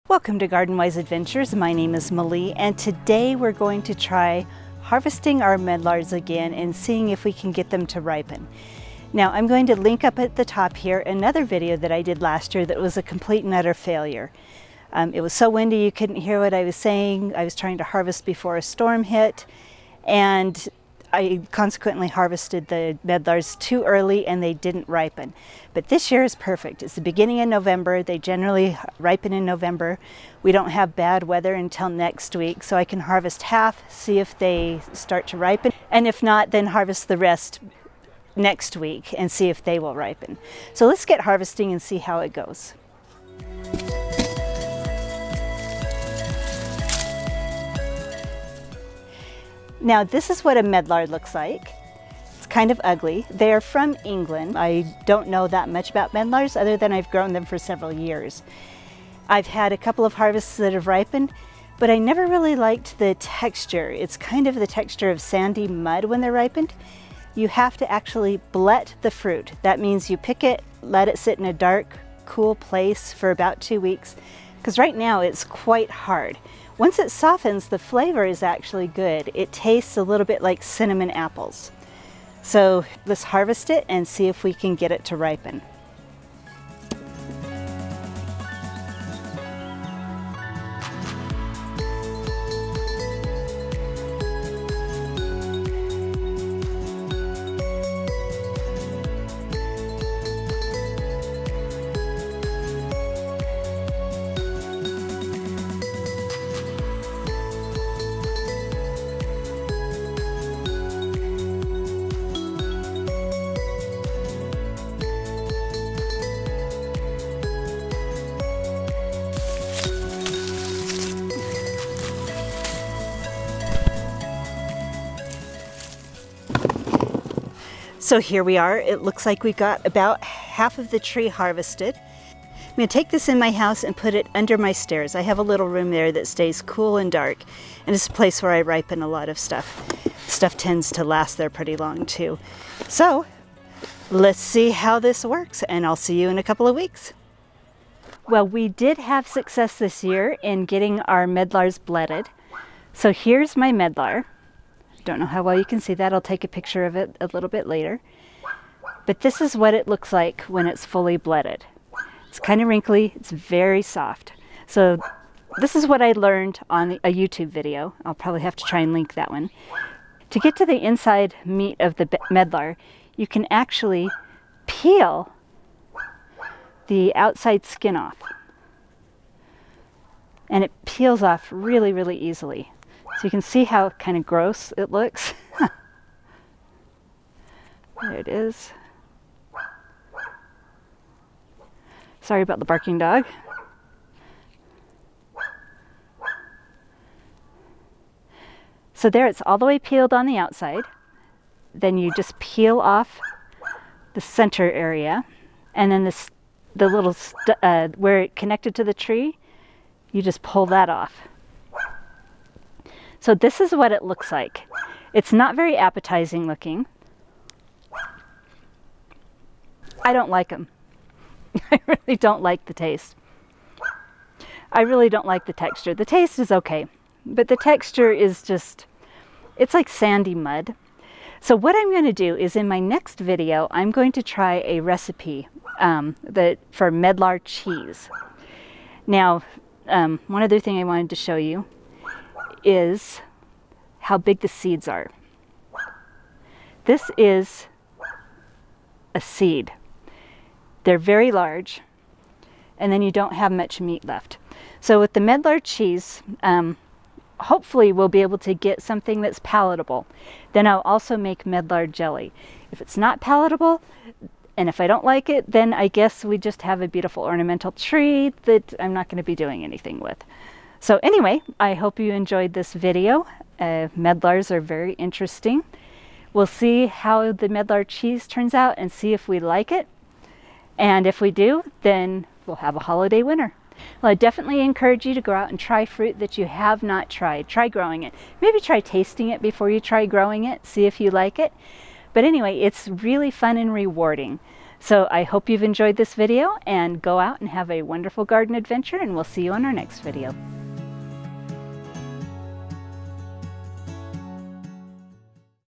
Medlar Harvest and Bletting
This year the Medlar harvest was a success, and I was able to properly blet them.